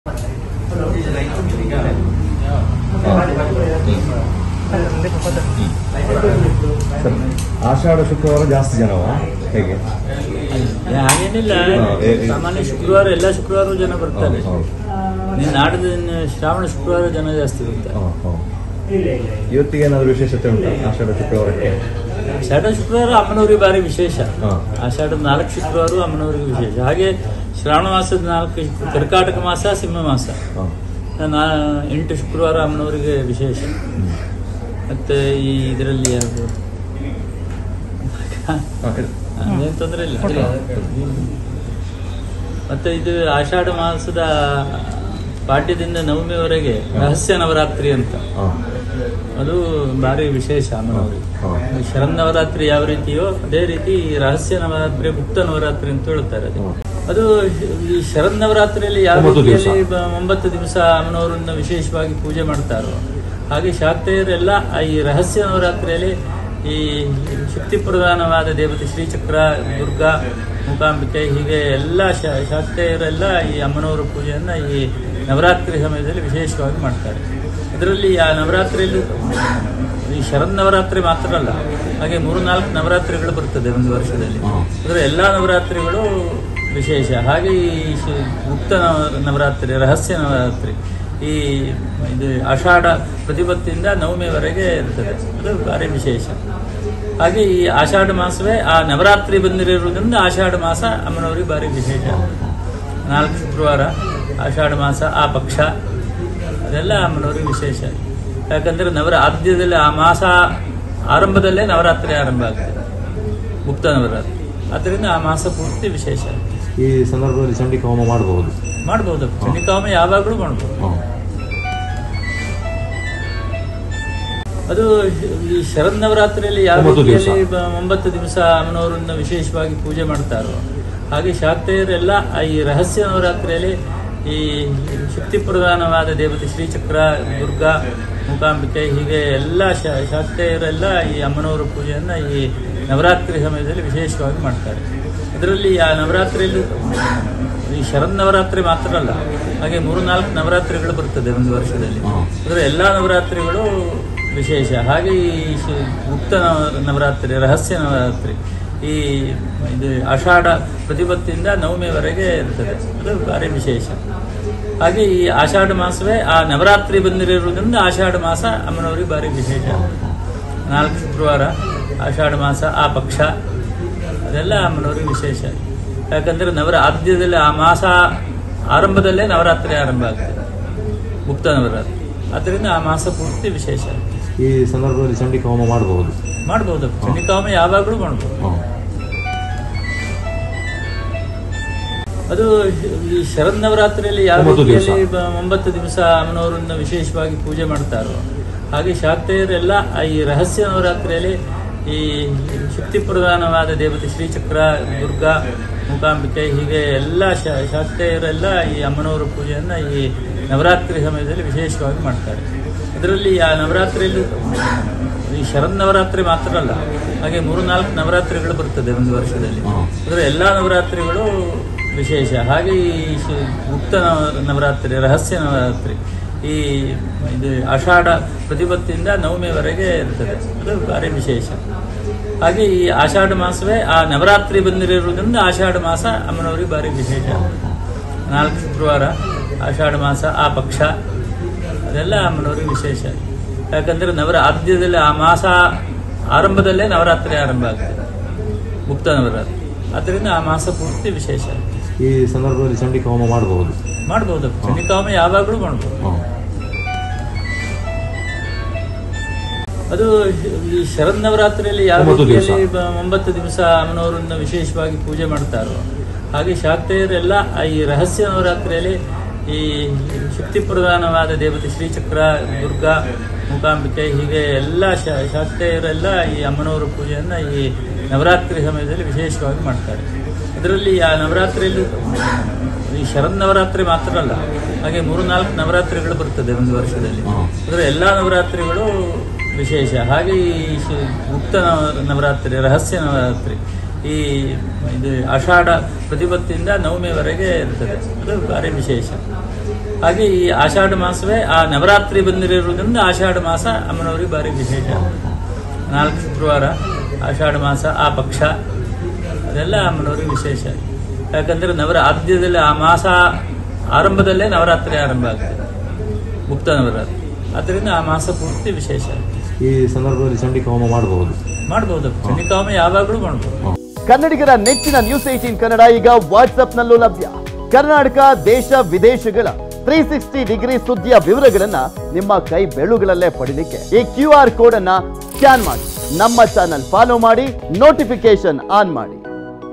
Vijayalakshmi Darshan Special Pooja At Kollur Temple | ಚಂಡಿಕಾಯಾಗ ಯಾವಾಗ ಬೇಕಾದ್ರೂ ಮಾಡ್ಬೋದು | n18v
News18 Kannada Live | ನ್ಯೂಸ್ 18 ಕನ್ನಡ ಲೈವ್
Karnataka | ಕರ್ನಾಟಕ
Kannada | ಕನ್ನಡ